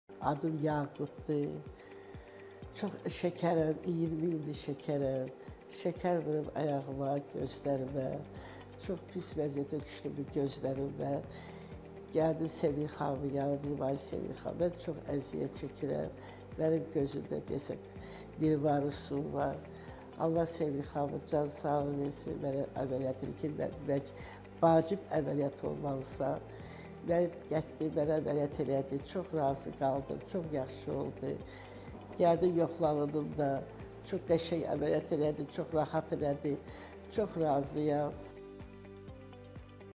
Katarakta əməliyyatı
Məmnun pasiyent Yaqut xanım